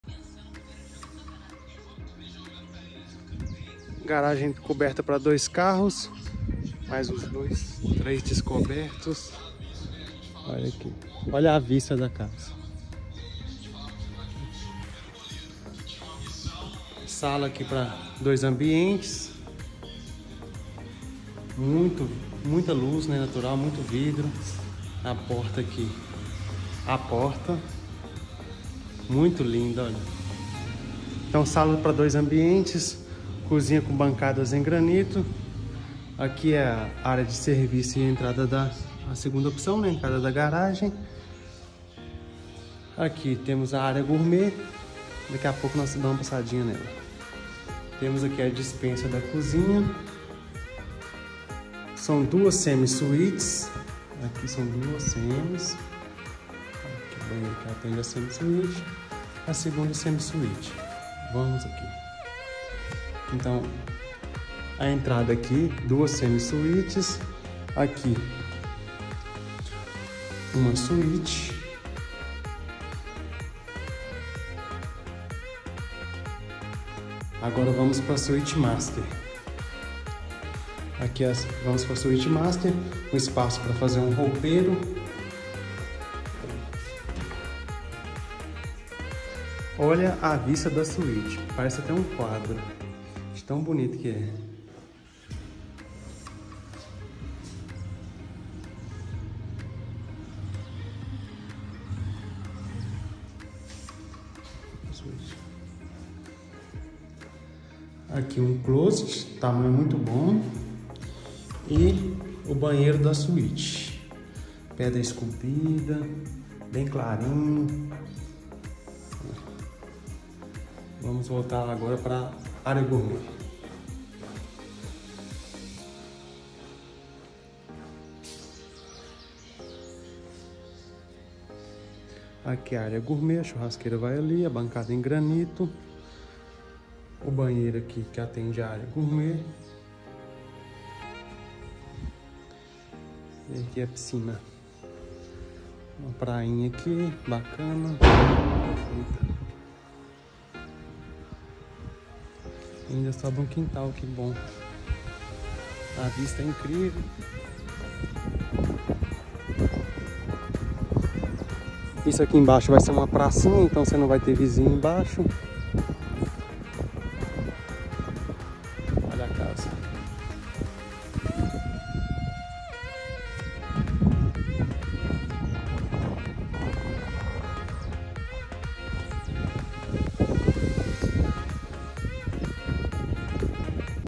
Linda casa no condomínio Monte Ville em fase final de obras 🚧
🚧 Linda casa em fase final de obras no condomínio Monte Ville em Lagoa Santa 🚧

Sendo
▫️ 4 quartos sendo 1 suíte simples 1 master com closet e 2 semi suítes
▫️Sala ampla para 2 ambientes rebaixamento em gesso
▫️Cozinha com bancadas em granito
▫️Despensa
▫️Área de serviço coberta
▫️Área gourmet com churrasqueira e bancadas em granito
▫️Lavabo de apoio
▫️Piscina
▫️Amplo quintal

🚘 4 Vagas
📐área do terreno 1.000m²
📐área construída 200,00 m²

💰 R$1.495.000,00

Informações e Visitas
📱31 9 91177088